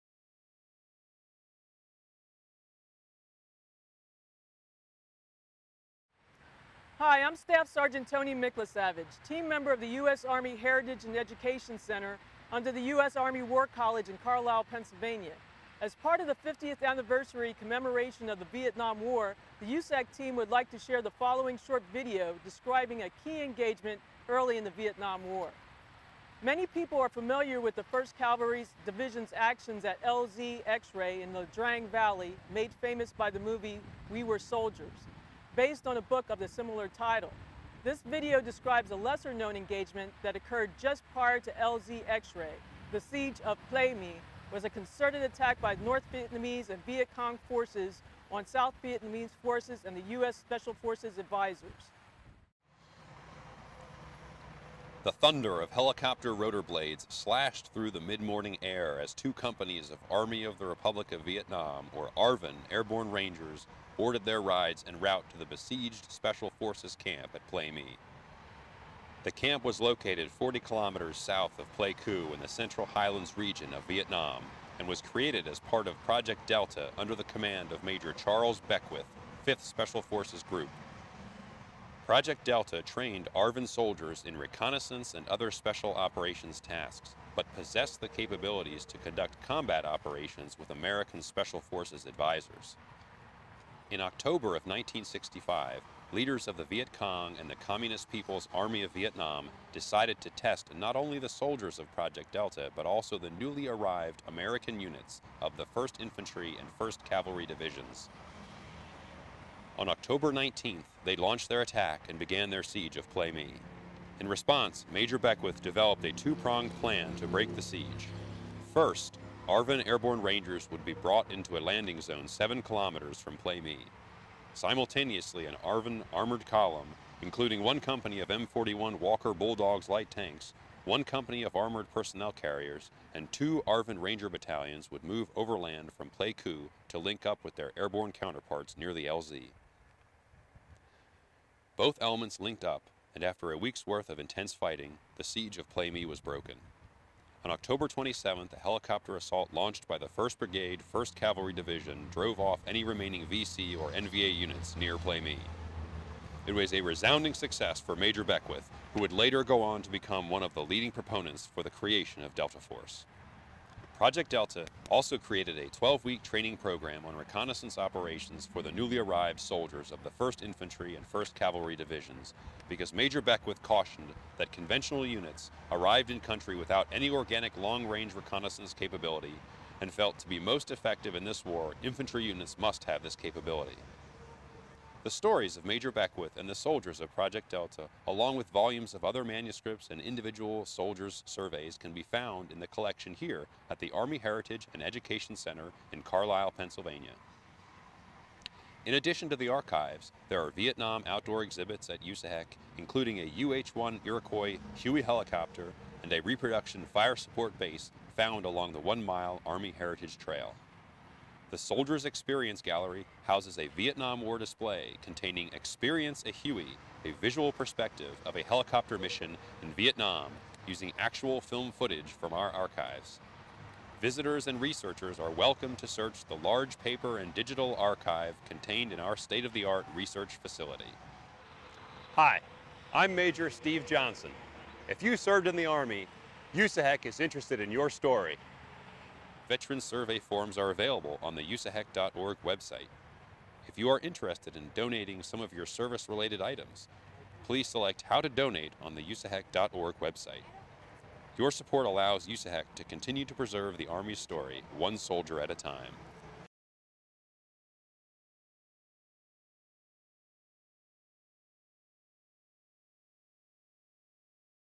Recreating History: The Siege of Plei Me
Using video footage and a Soldier story from the U.S. Army Heritage and Education Center collection, this video provides a brief overview of the Siege of Plei Me. It highlights the early history of the Vietnam War, and provides a brief amount of information at the end about the USAHEC and it's mission and functions.